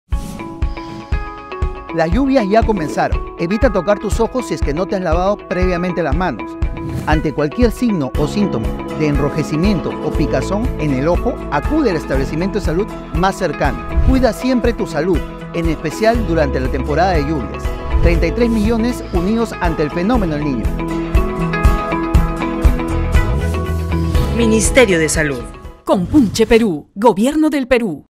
Recomendaciones para el cuidado de tu salud ocular
Te brindamos algunas recomendaciones para el cuidado de tu salud ante la temporada de lluvias